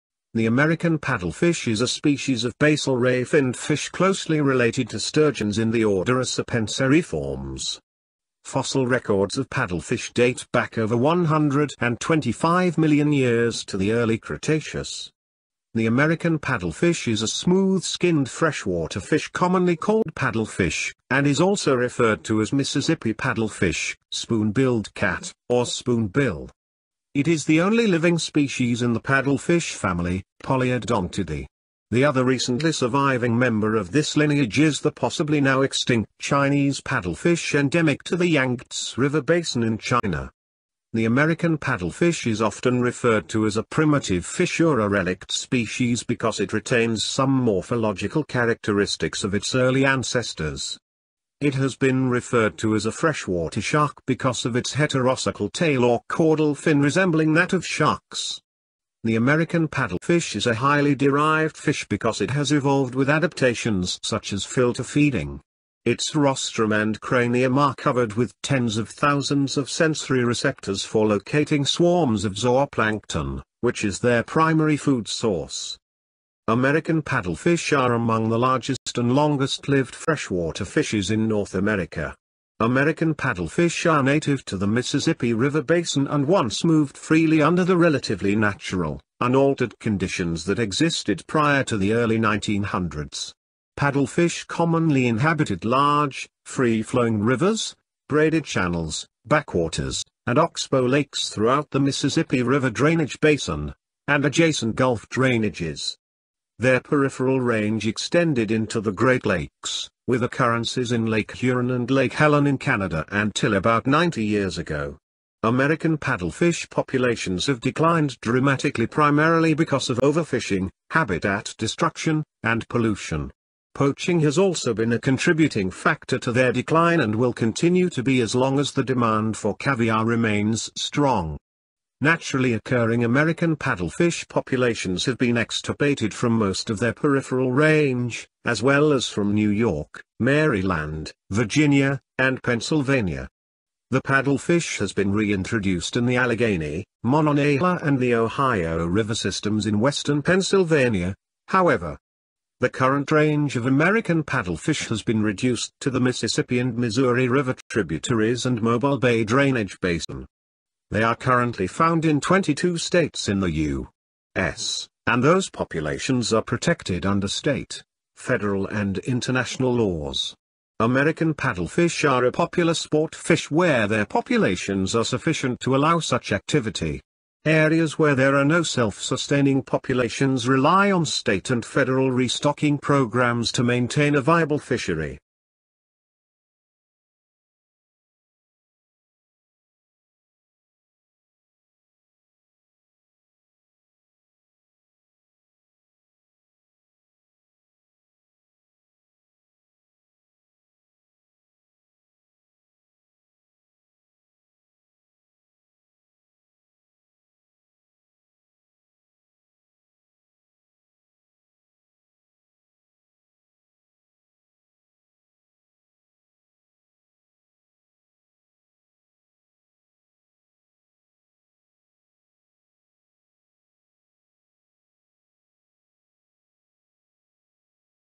1 American Paddle Fish
American paddlefish are closely related to sturgeons in the order Acipenseriformes, an order of basal ray-finned fishes that includes sturgeon and paddlefish, several species of which are now extinct.Paddlefish have a long fossil record which dates their first appearance approximately 125 million years ago. Fossils of a second extinct species, P. tuberculata, which dates back approximately 60 million years ago, were found in the Lower Paleocene Tullock Formation in Montana.